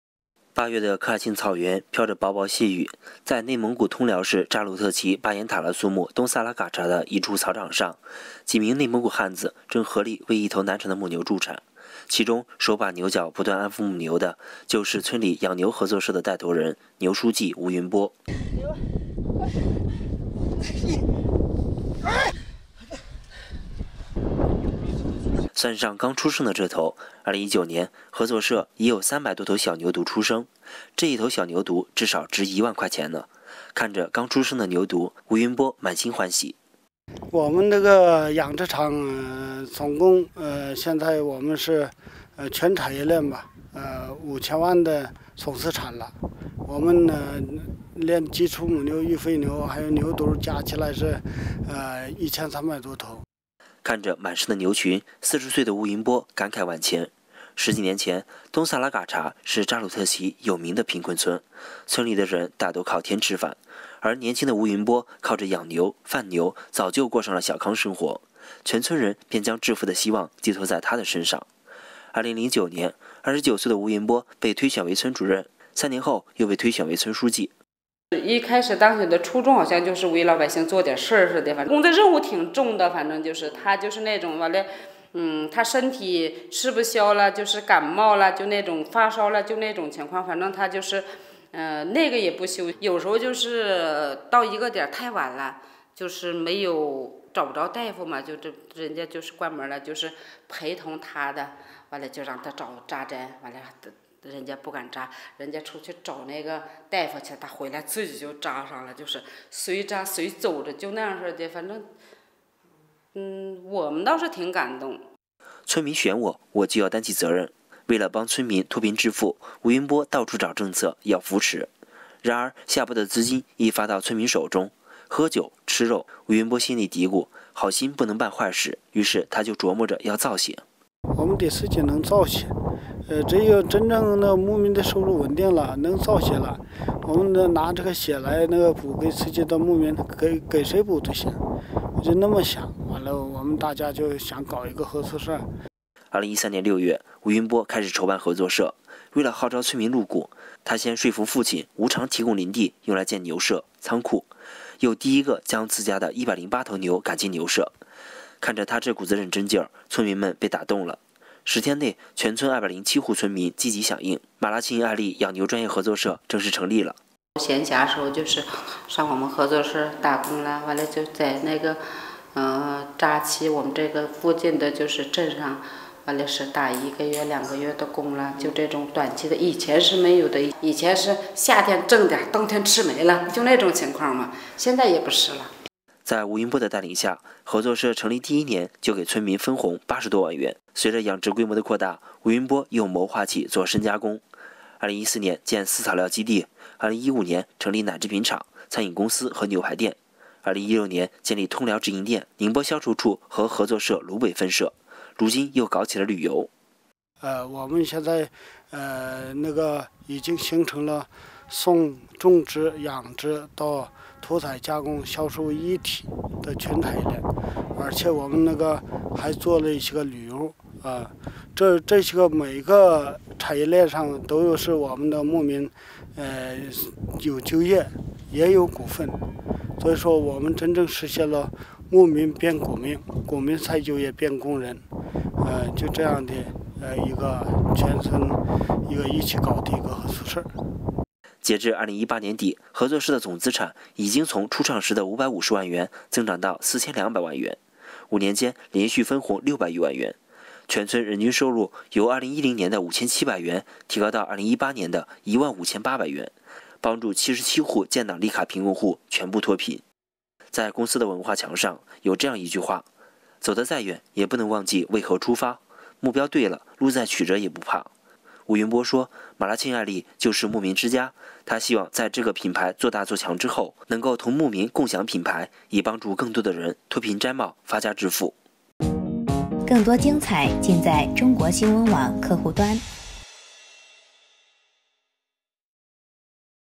【边疆党旗红】嘎查村做起牛产业 “牛书记”领路发牛财
八月的科尔沁草原飘着薄薄细雨，在内蒙古通辽市扎鲁特旗巴彦塔拉苏木东萨拉嘎查的一处草场上，几名内蒙古汉子正合力为一头难产的母牛助产，其中手把牛角不断安抚母牛的，就是村里养牛合作社的带头人，“牛书记”吴云波。